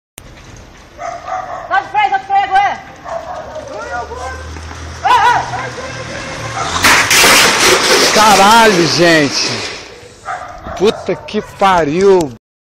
You gotta look both ways
Dude on bike gets hit by guy on motorcycle